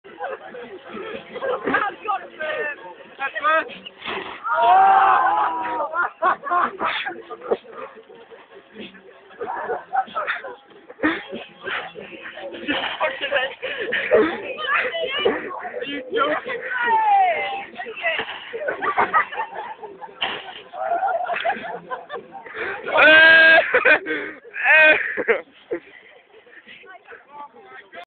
slip n slide piaf 06
piece of plastic, washin up liquid, cheap vodka plus a sample of magners... slip n slide!